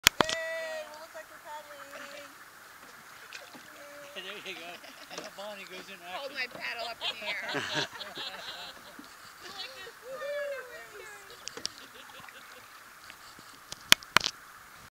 Gallinas Watershed Council cleaning up Gallinas Creek, Coastal Cleanup Day 2016
Gallinas Watershed Council volunteers in the Gallinas Goddess canoe on Gallinas Creek for Coastal Cleanup Day 2016